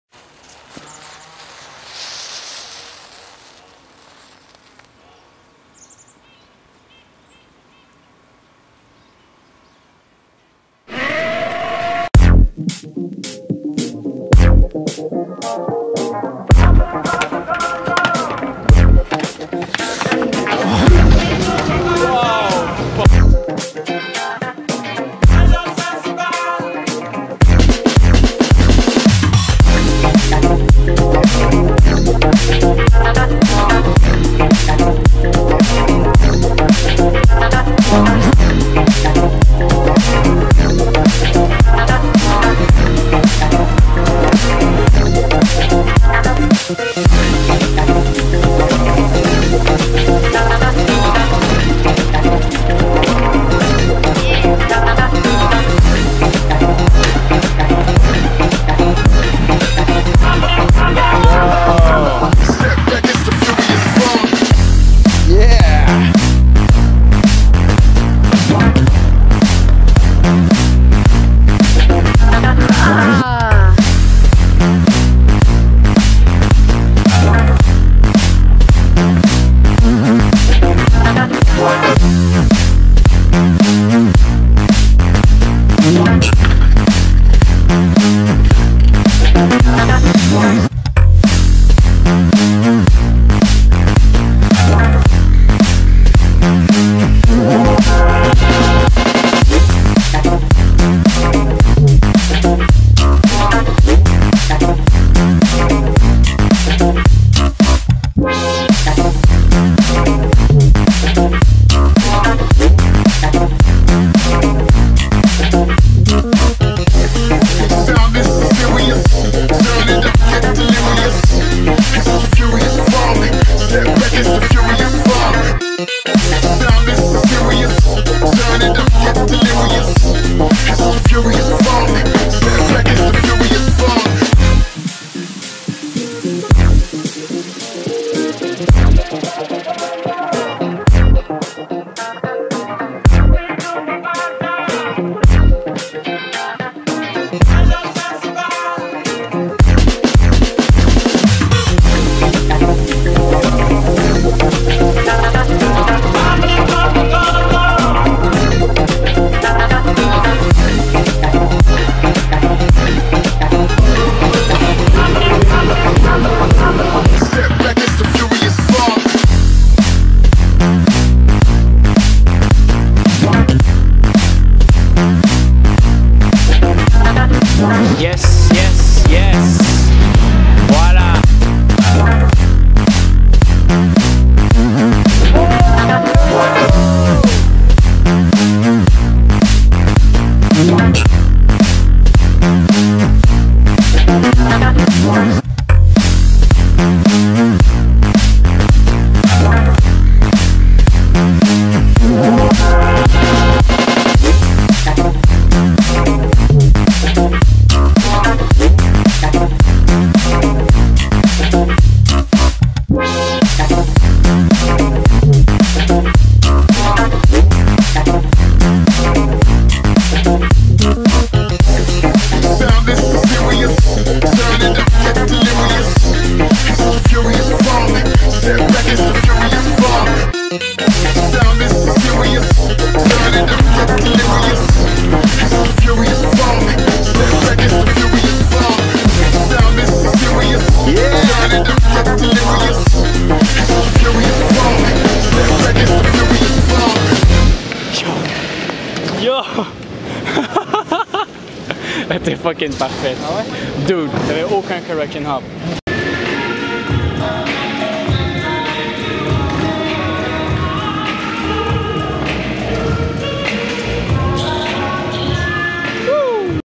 MTL Sessions 3.0 [Unicycling]
This edition with Nicolas, Loic, Kosma, Eli, Hugo and myself!

We ended up having to hit up the Taz due to bad weather!